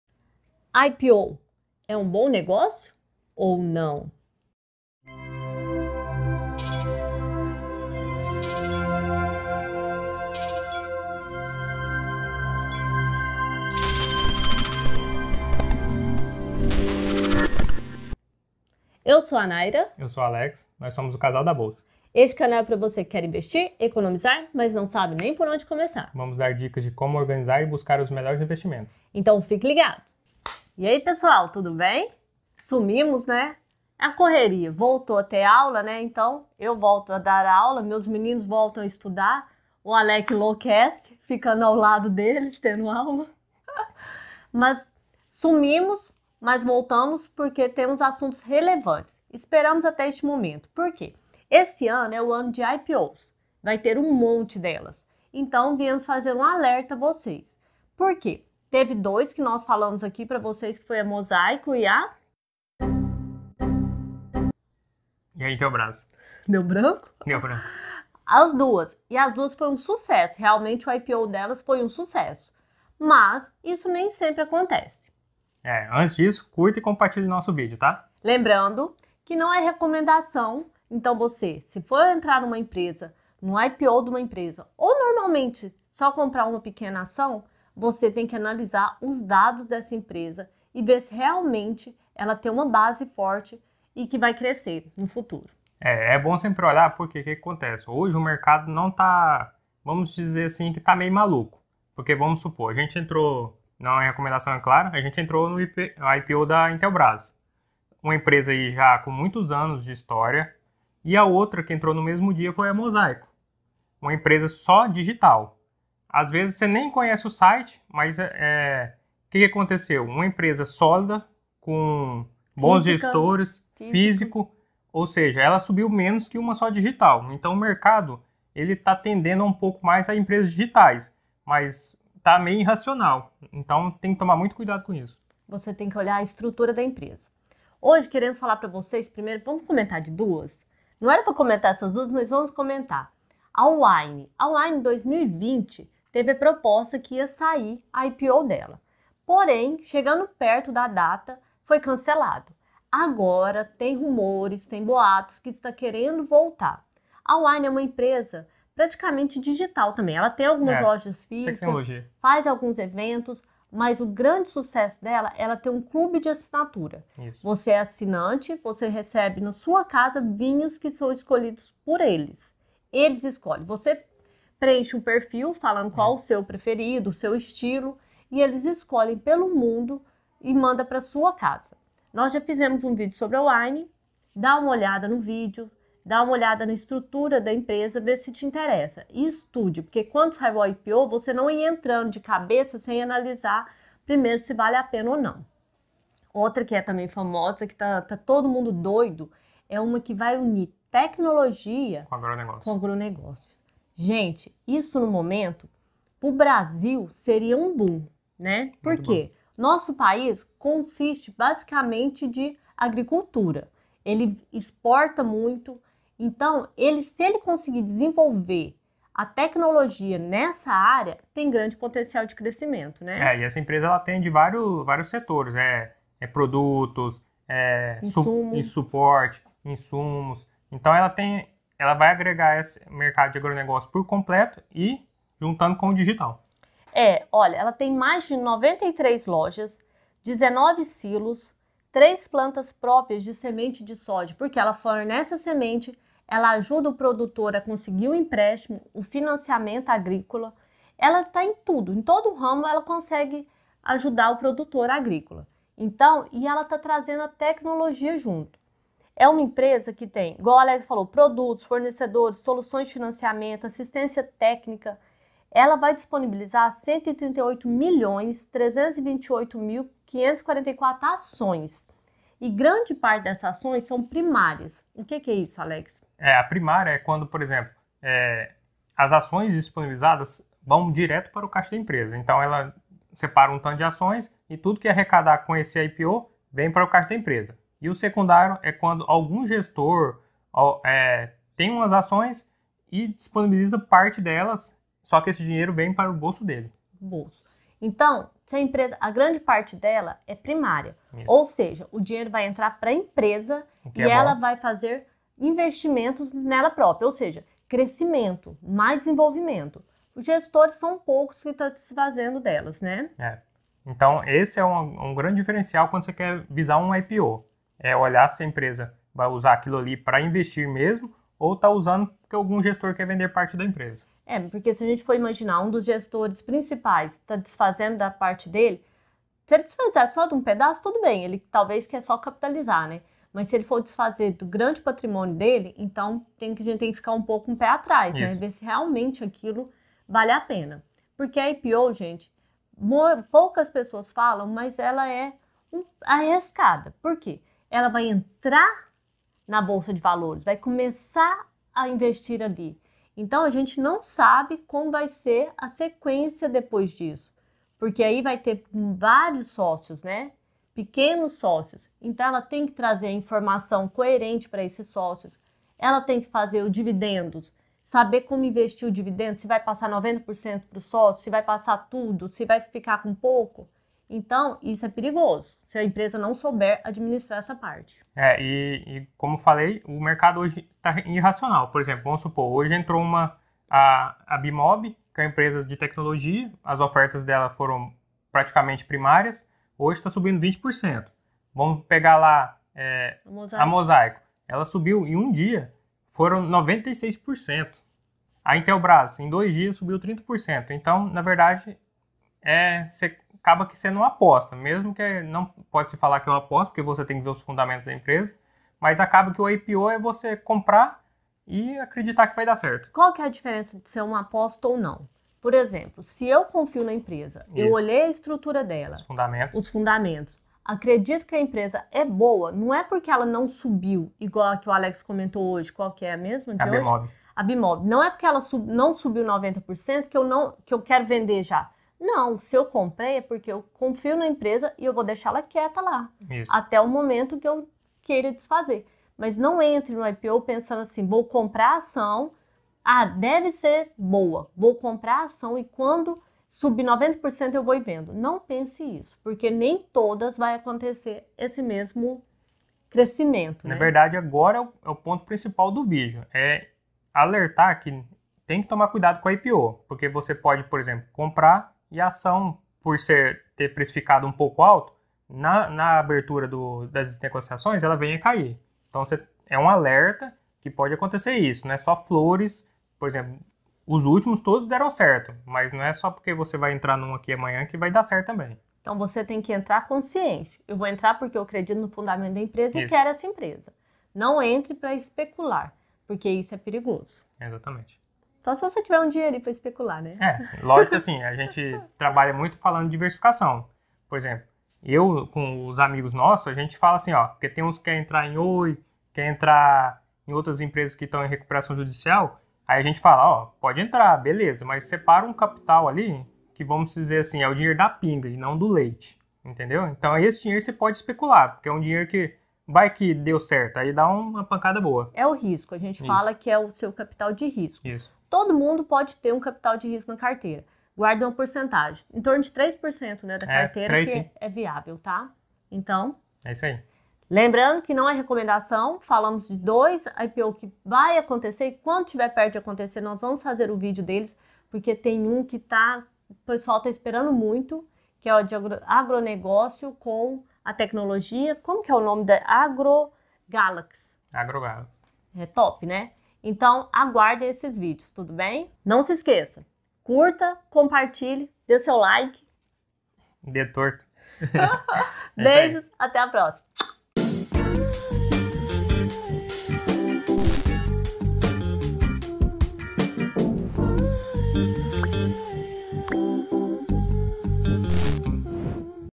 IPO quais seus riscos? 💥 PRÓS e CONTRAS 💥 INTELBRAS, AGROGALAXY, WINE
A Initial Public Offering (Oferta Pública Inicial), ou IPO, marca o início de uma empresa na Bolsa de Valores e por isso, é alvo de atenção por parte de muitos investidores. É a oportunidade ideal para desenvolver uma estratégia e buscar a lucratividade.

Nesse contexto, muitas empresas têm a ambição de crescer e se destacar no cenário nacional e internacional, e ter o capital aberto pode ser um ótimo caminho. Uma grande possibilidade para investir e obter lucros, sejam de longo ou curto prazo.

O Casal da Bolsa preza pela educação financeira e liberdade de escolha e expressão.

📱 Quer receber DICAS EXCLUSIVAS? Entra AGORA